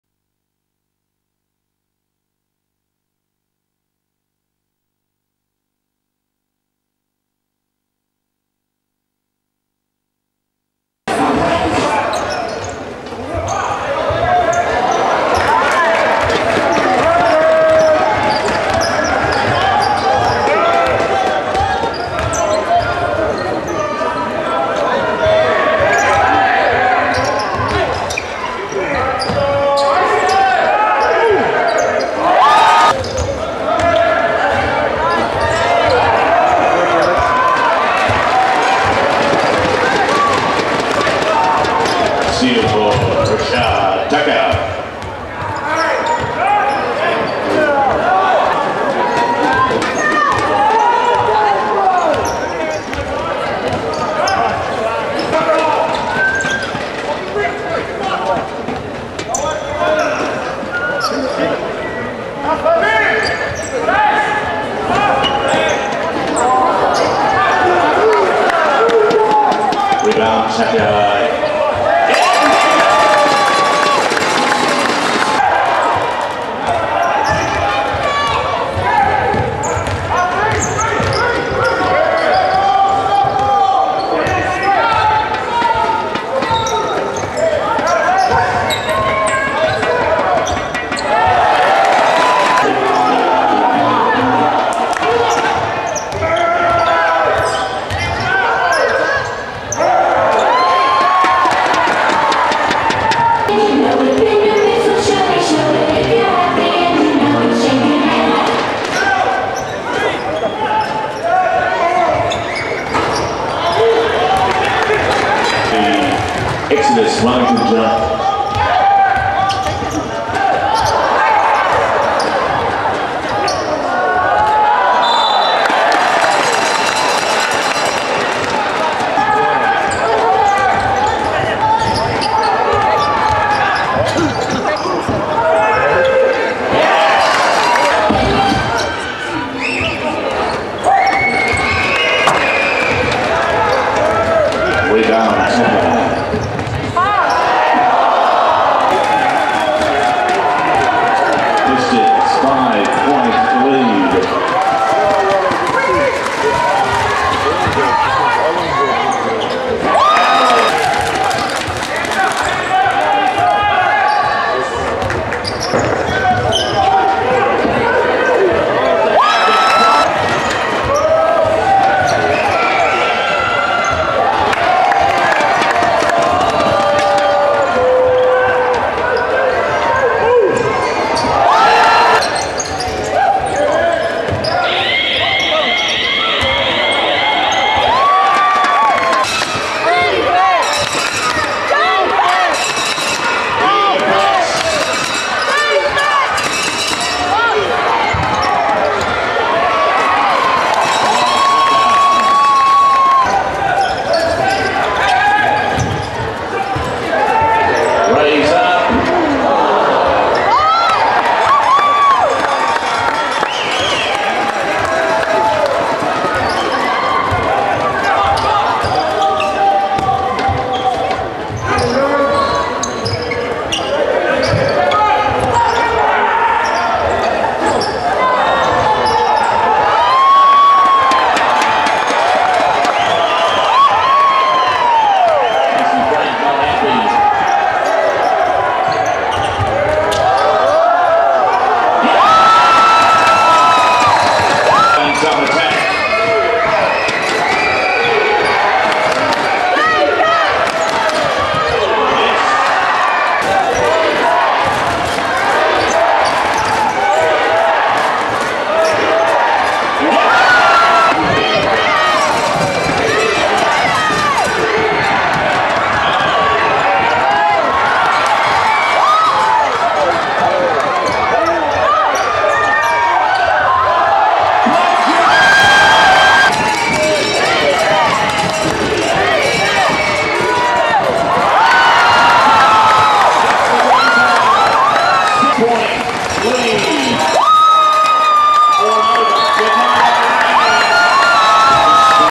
Rashad Tucker - New Zealand NBL Grand Final - Game 1.
Rashad Tucker - Waikato Pistons - 2010 NBL Grand Final - Game 1.
 3/5 - 7 points - 13 rebounds - 8 assists - 33mins.
Grand Final Ave: 12 points - 8 rebs - 5 assists.